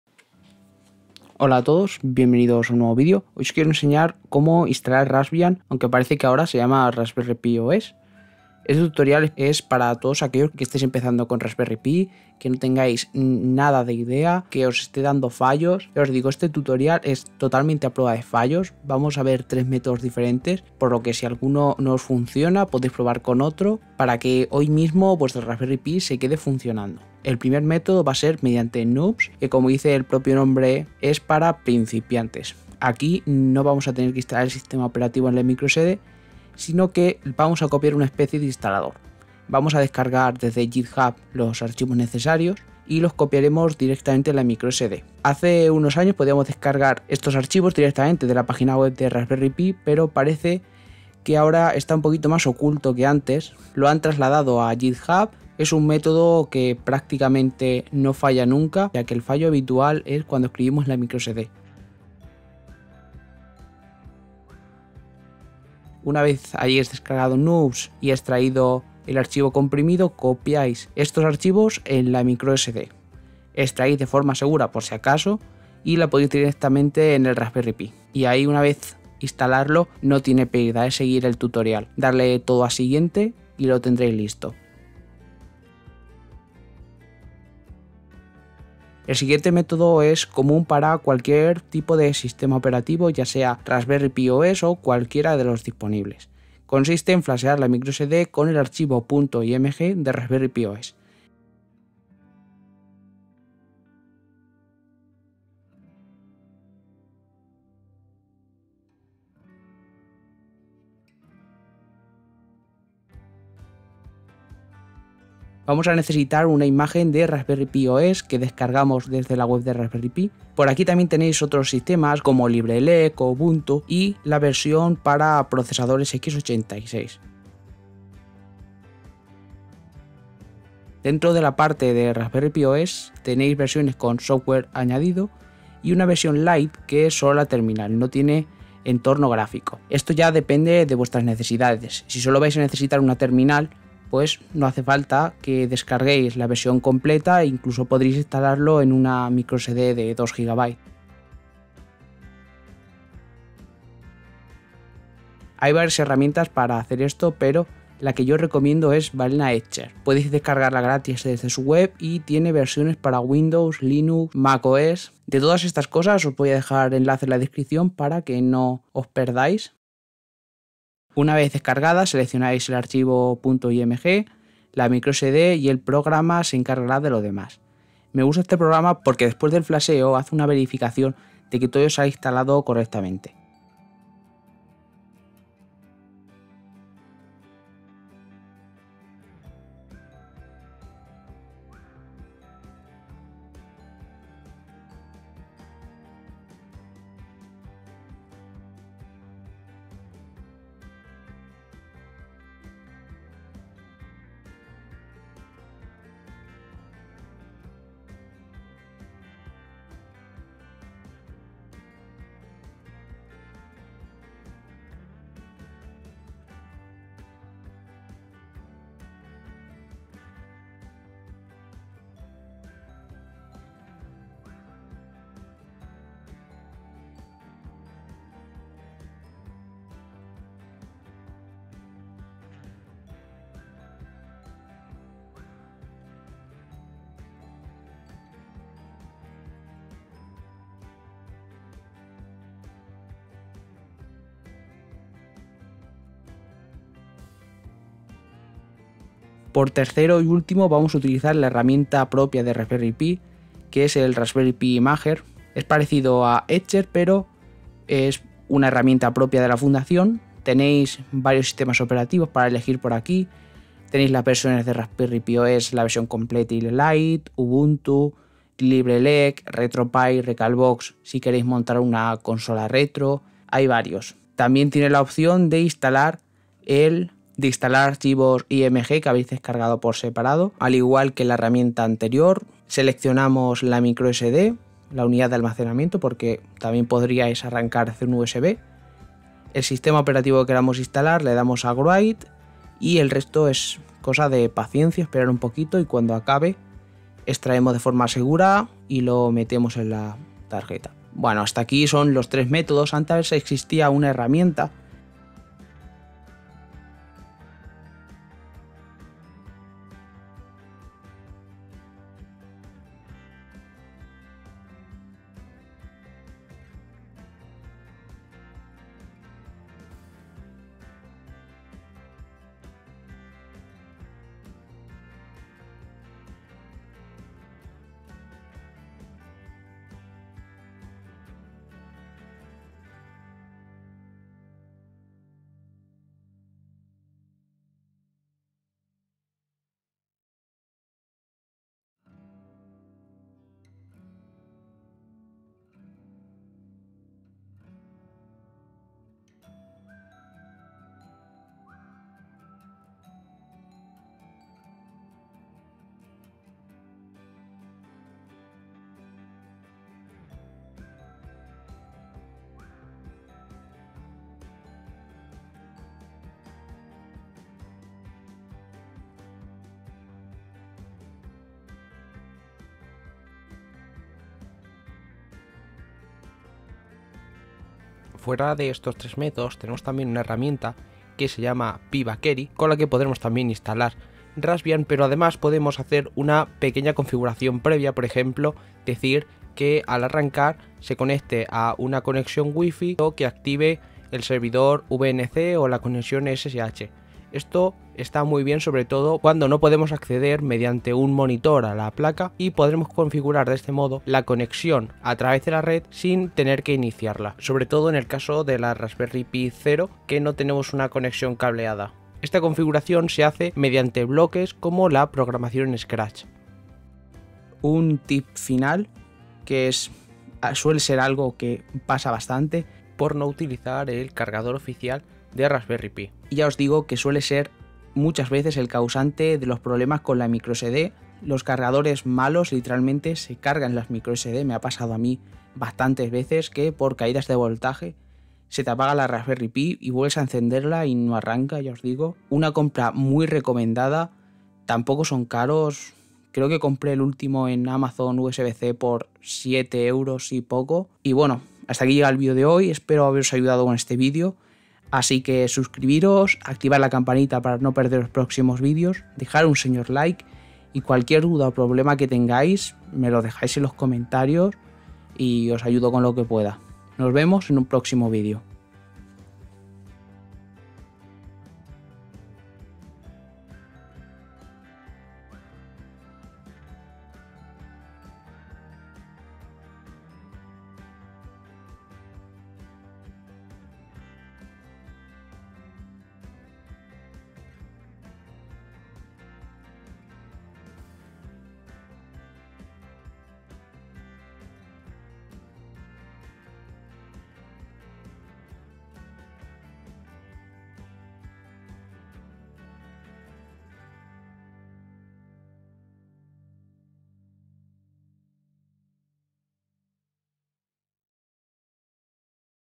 TUTORIAL Instalar Raspberry Pi OS / Raspbian desde 0, 3 MÉTODOS ESPAÑOL Cualquier sistema operativo
Instalar Raspberry Pi OS / Raspbian para principiantes 3 MÉTODOS EN ESPAÑOL, válido para cualquier sistema operativo

Raspberry Pi 4
Fuente Alimentación Oficial
Raspberry PI 3A+